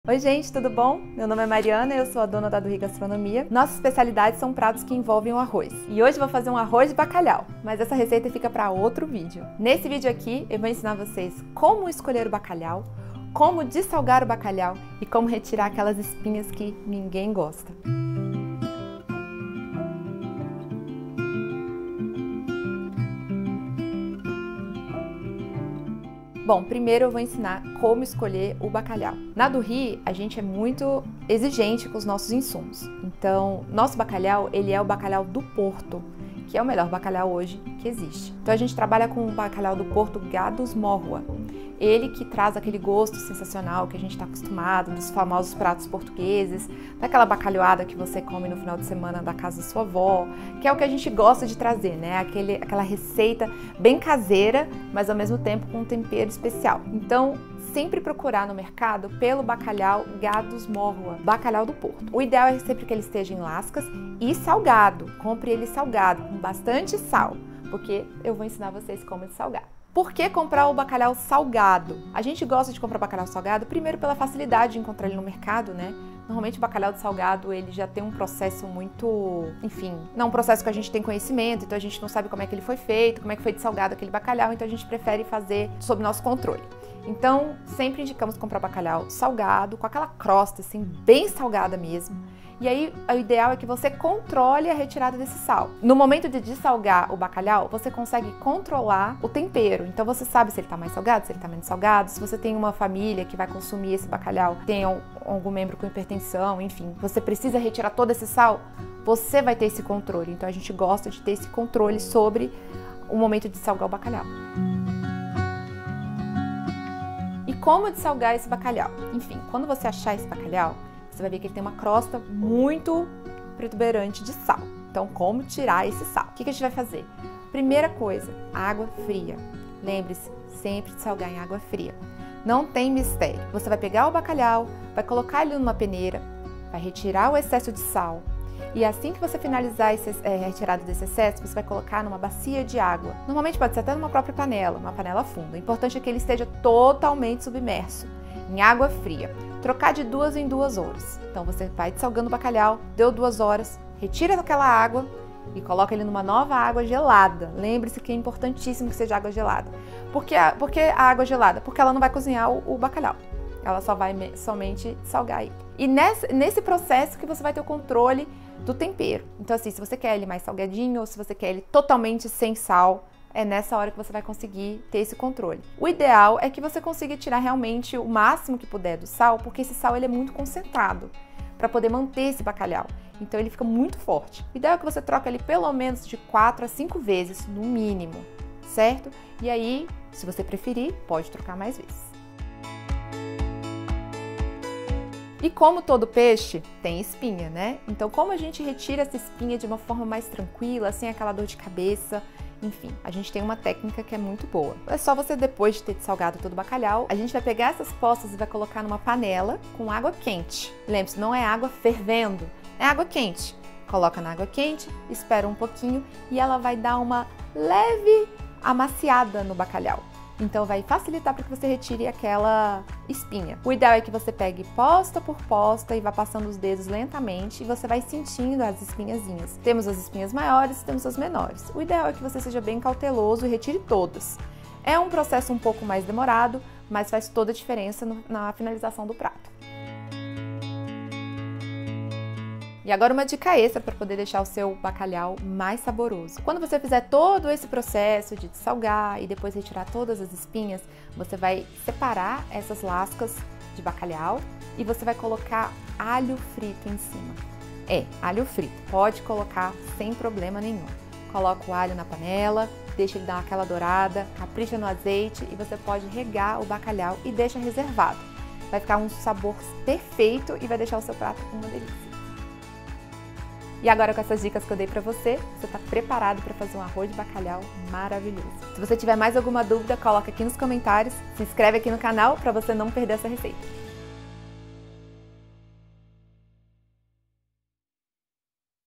DICAS PARA UMA RECEITA DE BACALHAU PERFEITA | GPSTV & Du Riz Gastronomia
O bacalhau é um prato típico da culinária portuguesa, que quando preparada da forma correta, os sabores são únicos e inesquecíveis. Quer saber dicas para o bacalhau perfeito?

No episódio de hoje da GPS|TV, a chef Mariana Jordão, dona da Du Riz Gastronomia, mostra conselhos para o preparo ideal do peixe, passando pela escolha do bacalhau, e mostrando como dessalgar e retirar as espinhas!

Seguindo os ensinamentos desse vídeo, qualquer prato com a iguaria vai surpreender o paladar de todos à mesa. E aí, ficou com vontade? Aperte o play e, no final, curta, deixe um comentário e compartilhe essas informações preciosas com os amigos!

00:00​​​ Intro
00:26 Como escolher o Bacalhau
01:09 Por que comprar salgado
01:57 Como dessalgar
03:15 Como tirar as espinhas
04:02 Dica extra
04:32 Encerramento